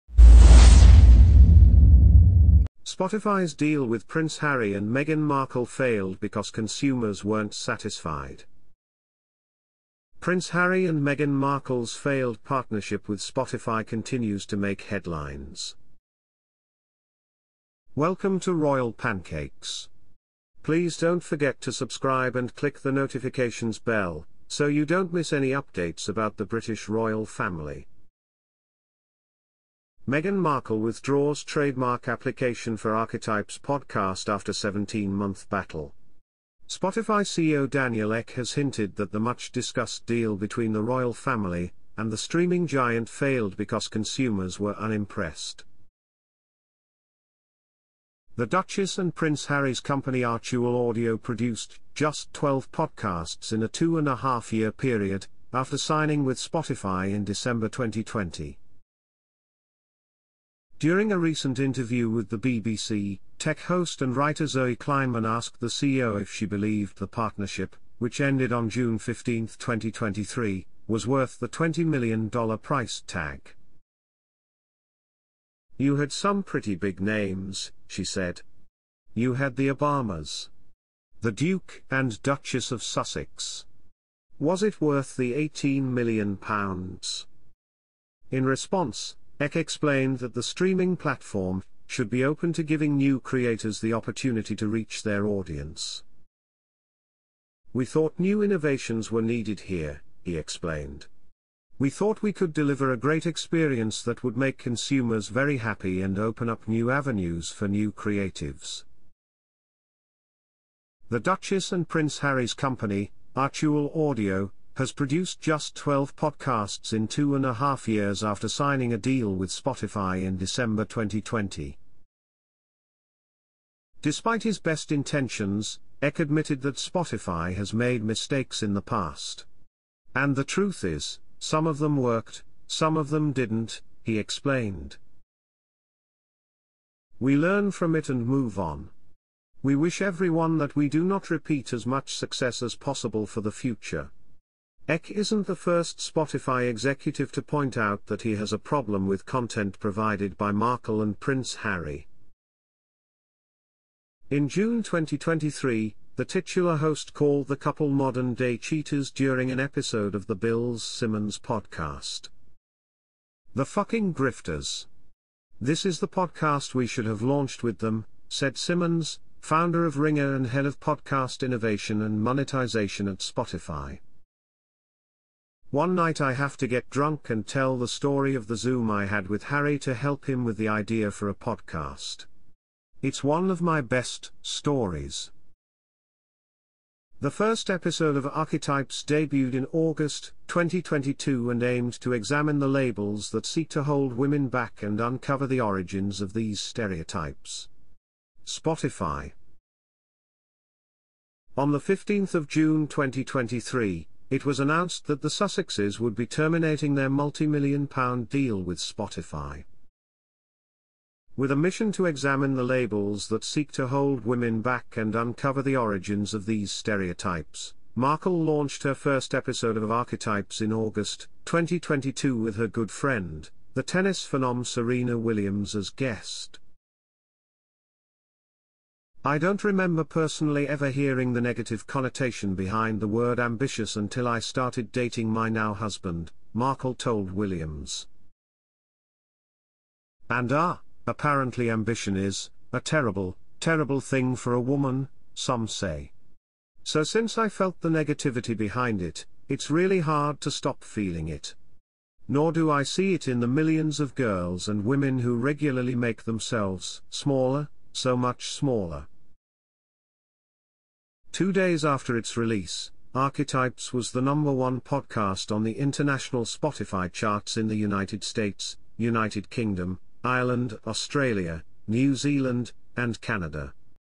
Harry frustrated as Spotify cancels their deal with him claiming that want nothing to do with Meghan
Spotify's deal with Prince Harry and Meghan Markle failed because 'consumers' weren't 'satisfied'.

Prince Harry and Meghan Markle's failed partnership with Spotify continues to make headlines.
Meghan Markle withdraws trademark application for 'Archetypes' podcast after 17-month battle. Spotify CEO Daniel Ek has hinted that the much-discussed deal between the royal family and the streaming giant failed because consumers were unimpressed.

The Duchess and Prince Harry's company Archewell Audio produced just 12 podcasts in a two-and-a-half year period, after signing with Spotify in December 2020.

During a recent interview with the BBC, tech host and writer Zoe Kleinman asked the CEO if she believed the partnership, which ended on June 15, 2023, was worth the $20 million price tag.

“You had some pretty big names,” she said. “You had the Obamas. The Duke and Duchess of Sussex. Was it worth the £18m?In response, Ek explained that the streaming platform should be open to giving new creators the opportunity to reach their audience.